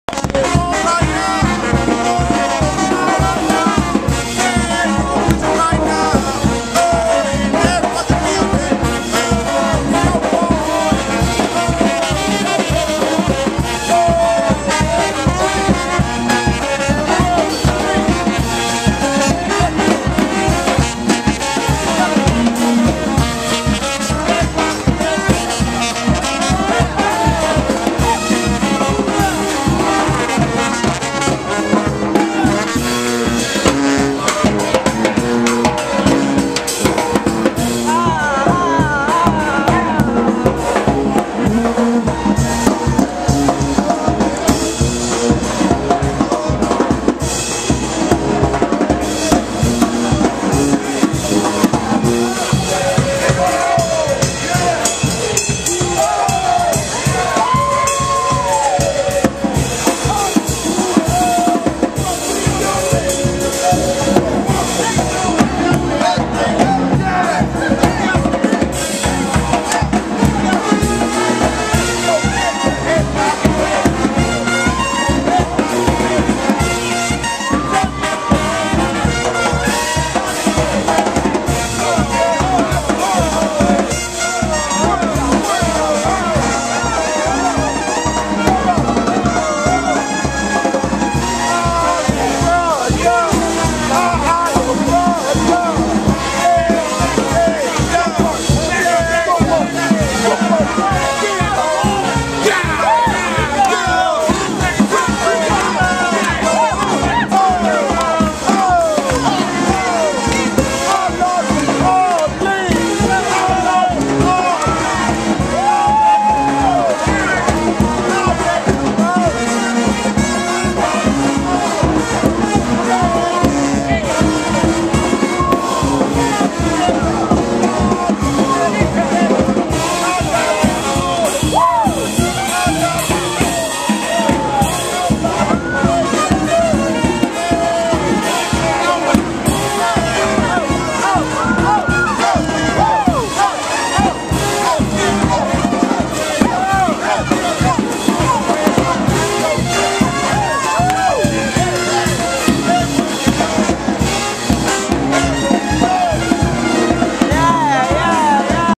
Shantrelle + Bourbon + Brass Band = Buck jump time!
I can't help myself...I'm from New Orleans. What else is one to do when one hears music like this in the streets but to second line.